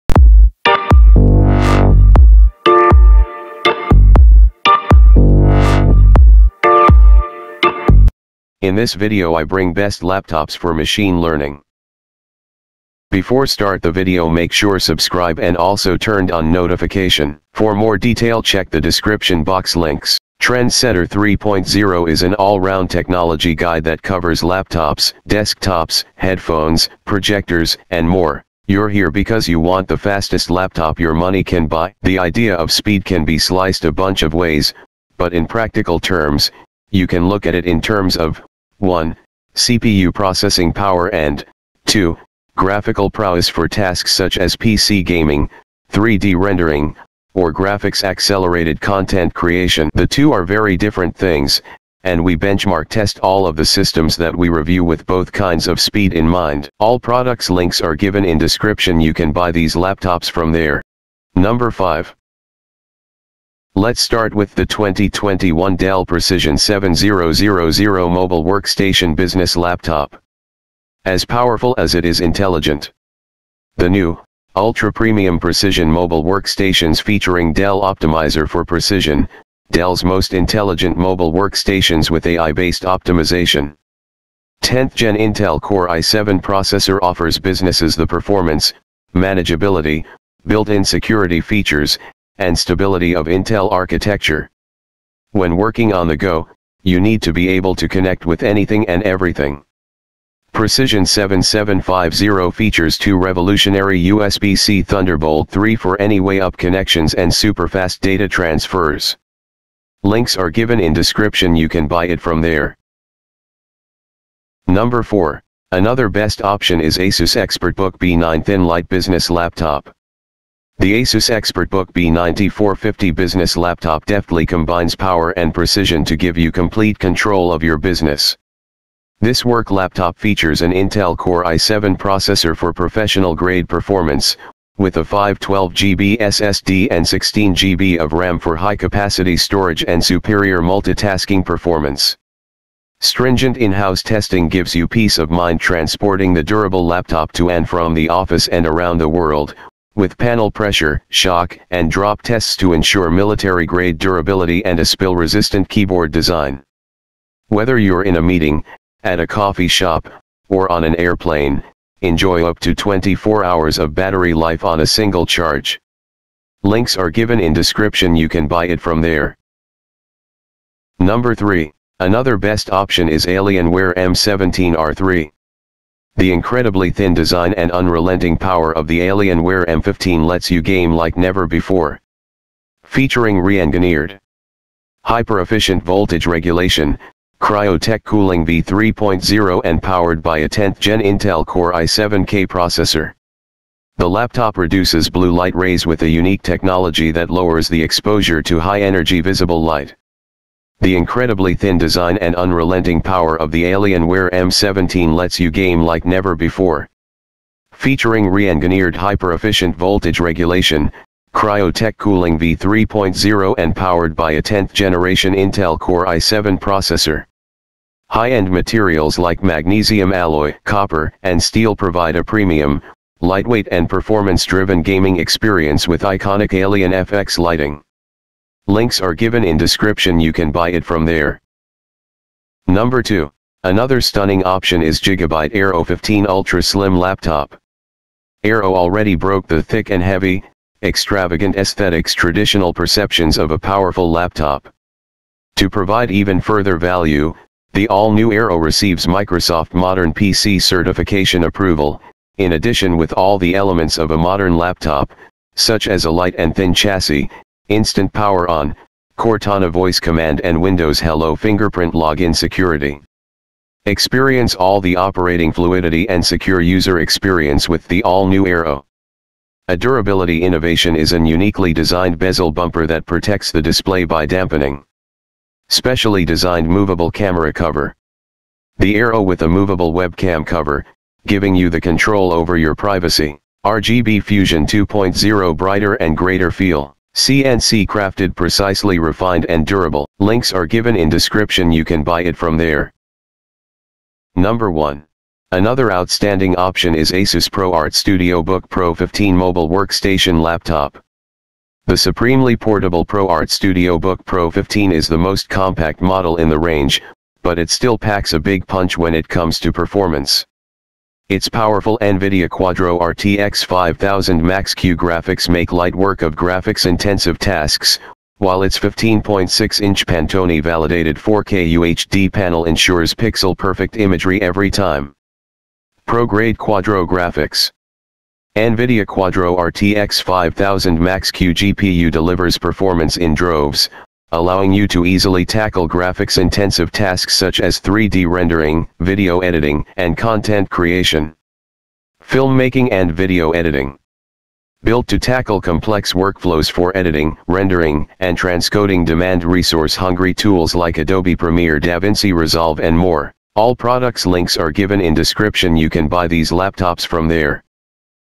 Top 5: Best Laptops for Machine Learning and Deep Learning 2021
Are you looking for the Best Budget Laptops for Machine Learning and Deep Learning 2021... for more details check out the link given below...

✅5. 2021 DELL Precision 7000 Mobile Workstation Business Laptop
[🛒AMAZON International Link]
[🛒AMAZON india Links]

: High-end materials like magnesium alloy, copper, and steel provide a premium,
: lightweight and performance-driven gaming experience with iconic AlienFX lighting
: Our fastest panel yet offers 300Hz 300nits 3ms Full HD for smooth high-speed motion graphics
: Alienware Cryo-Tech optimizes component cooling
: maximizes overall performance and keeps your laptop cool to the touch
: The new Alienware m17’s standard display features a 300Hz 3ms Full HD panel.
: At just 4.65lb and under 20.5mm thin

✅4. ASUS ExpertBook B9 Thin Light Business Laptop
[🛒AMAZON International Link]
[🛒AMAZON india Links]

: Professional-grade performance:
: Intel Core i7-10510U Processor
: 512GB SSD, 16GB RAM, and Windows 10 Pro
: Extreme military-grade durability: Panel pressure, shock, and drop tested to ensure resilience,
: 180-degree hinge and I/O ports (1x USB 3.2 Type-A (Gen 2),
: Lasts longer and charges faster
: Up to 24 hours of battery life on a single charge and recharges up to 60% capacity in 45 minutes
: Unrivaled visuals and security:
: 14-inch Full HD anti-glare NanoEdge display with built-in protection of a webcam privacy shield
: IR camera, Kensington lock, TPM, and fingerprint sensor
: Lightweight and portable:
: 2.2-pound lightweight magnesium-lithium alloy chassis and ultrathin 0.6-inch design
: Great for Work Anywhere: Spill-resistant keyboard, backlit keyboard, Harman Kardon speakers,

✅3. Alienware m17 R3 17.3 inch FHD Gaming Laptop
[🛒AMAZON International Link]
[🛒AMAZON india Links]

: Our fastest panel yet offers 300Hz 300nits 3ms Full HD for smooth
: high-speed motion graphics
: Alienware Cryo-Tech optimizes component cooling, which maximizes overall performance and keeps your laptop cool to the touch

✅2. Gigabyte AERO 15 Ultra-Slim Laptop
[🛒AMAZON International Link]
[🛒AMAZON india Links]

: 15.6" 0.11 inch ultra-thin bezel
: Samsung UHD 3840x2160 AMOLED display
: X-Rite Pantone Certified for color calibration
: NVIDIA GeForce RTX 2070 with MAX-Q design GDDR6 8GB Supports NVIDIA Optimus technology
: Chipset: Mobile Intel HM370 Express Chipset
: 16GB DDR4 2666MHz Samsung DRAM module
: M.2 NVMe PCIe 512GB Intel 760P SSD
: Microsoft Azure AI | all Intel inside | Intel Thunderbolt 3
: Killer Ethernet E2600/ Killer Wi-Fi 6 AX1650 (powered by Intel)
: 94.24Wh 8.5hrs Long battery life | per-key RGB backlit programmable keyboard

✅1. ASUS ProArt StudioBook Pro 15 Mobile Workstation Laptop
[🛒AMAZON International Link]
[🛒AMAZON india Links]

: 15.6'' UHD (3840*2160) NanoEdge matte display; 100% Adobe RGB
: Pantone validated color accuracy; Delta E 1.5
: Intel Core i7-9750H with Windows 10 Professional
: NVIDIA Quadro RTX 5000 with 16GB GDDR6 VRAM - RTX Studio Ready
: Fast storage and memory featuring 1TB + 1TB (2TB total)
: PCIe NVMe SSD  Hyper Drive and 48GB DDR4 Memory
: Extensive connectivity with HDMI 2.0, RJ45, USB-C 3.2 (Gen 2), USB-A 3.2 (Gen 2)
: Wi-Fi 6 Gig+ (802.11ax) latest Wi-Fi standard + Dual-band Bluetooth 5.0
: 18.9mm thin profile in magnesium-alloy at portable 4.37lbs

These products in this video belong to the website owner and the product owner.